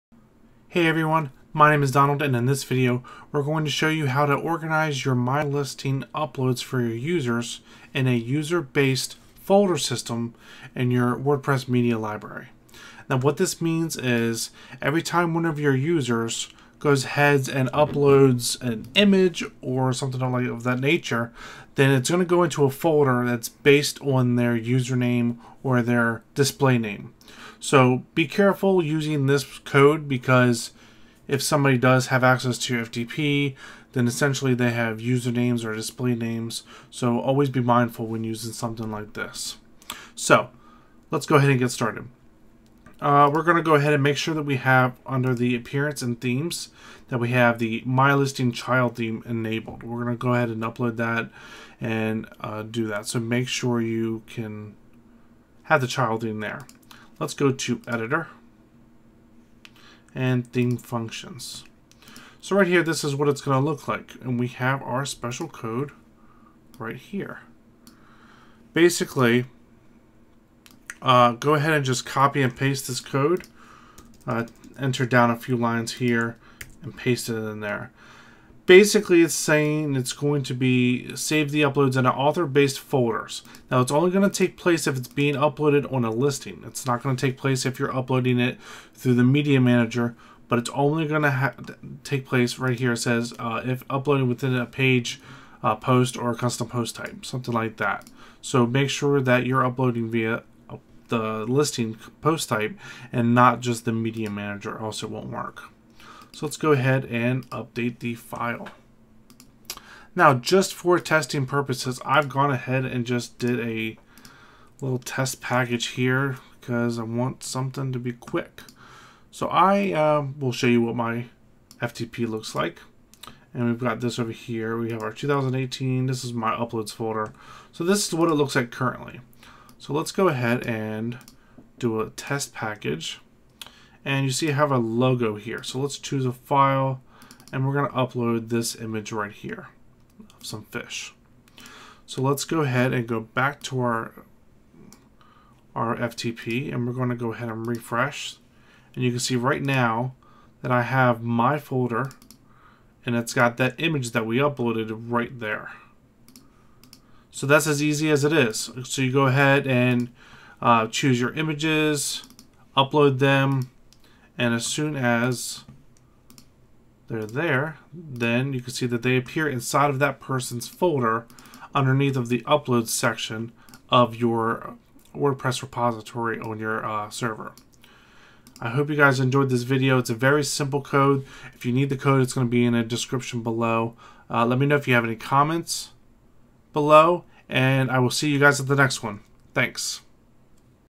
How to Create Author Based Folders in Your WordPress Uploads Folder with MyListing Theme
Be careful when using author based codes for your FTP as it could expose username/display names.